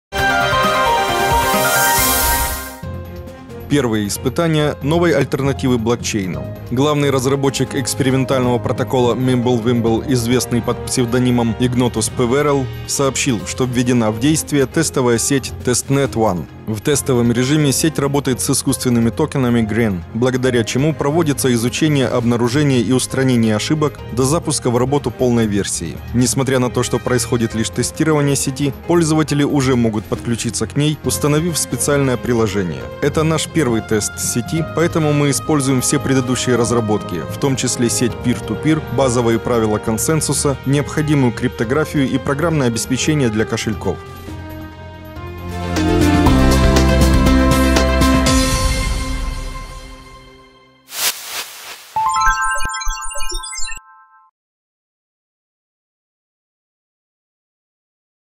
Создатели Mimblewimble запустили тестовую сеть
Первые испытания новой альтернативы блокчейну. Главный разработчик экспериментального протокола Mimblewimble, известный под псевдонимом Игнотус Певерелл (имена от Гарри Поттера), сообщил, что введена в действие тестовая сеть - testnet1.  В тестовом режиме сеть работает с искусственными токенами Grin, благодаря чему проводится изучение, обнаружение и устранение ошибок до запуска в работу полной версии. Несмотря на то, что происходит лишь тестирование сети, пользователи уже могут подключиться к ней, установив специальное приложение. “Это наш первый тест сети.  Поэтому мы испытываем все предыдущие разработки, в том числе сеть Peer-to-Peer, базовые правила консенсуса, необходимую криптографию и программное обеспечение для кошельков”, — говорит  разработчик.